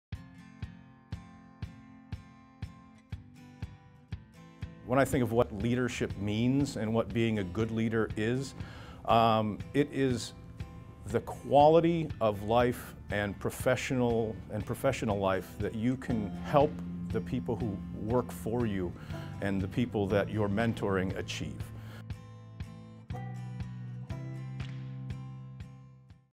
Steve Stroud on Leadership
Our VP of Marketing and Strategy, Steve Stroud shares his IntersportInsights on being a good leader in business.